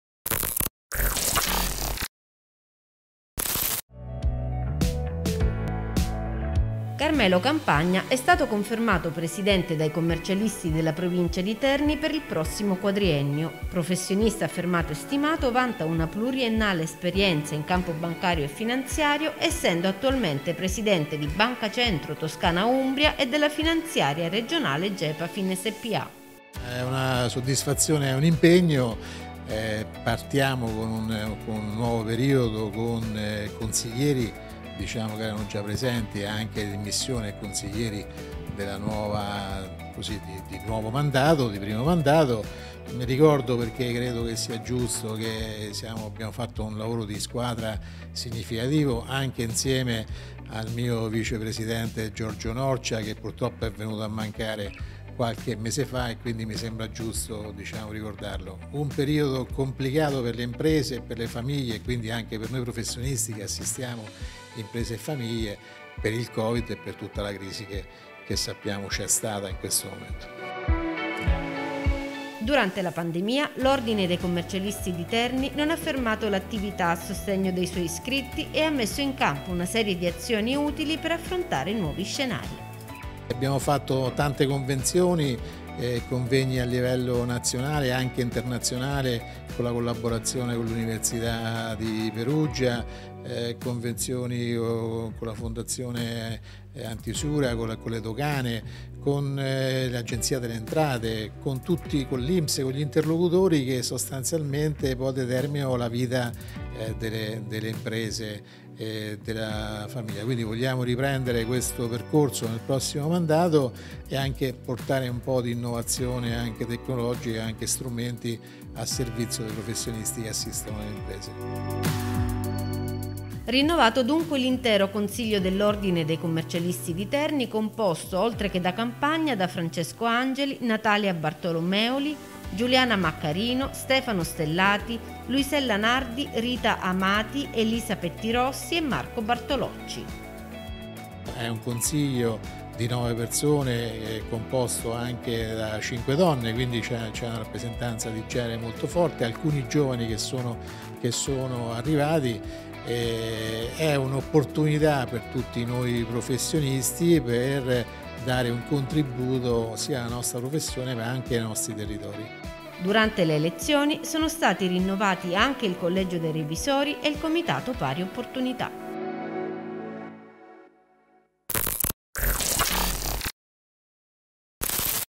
Carmelo Campagna confermato presidente dei commercialisti di Terni [UMBRIA NEWS]
Carmelo Campagna è stato confermato presidente dai commercialisti della provincia di Terni per il prossimo quadriennio. Si sono infatti svolte le elezioni per il rinnovo del Consiglio dell’Ordine dei dottori commercialisti ed esperti contabili della provincia di Terni.
Campagna vanta una pluriennale esperienza in campo bancario e finanziario essendo attualmente presidente di Banca Centro Toscana-Umbria e della finanziaria regionale Gepafin Spa.